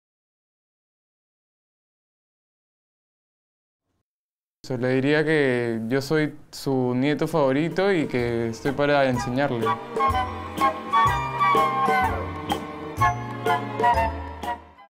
¿Le enseñarías a tu abuelo a usar un smartphone? Abuelos Smart - Telefónica
Nada como disfrutar cada momento de nuestras vidas. La Vida te Llama, un movimiento que tiene el fin de impulsar una serie de consejos proporcionados por todos los peruanos, para celebrar la vida.

La vida te llama es una iniciativa de Telefónica del Perú.

¡Te invitamos a conocer más!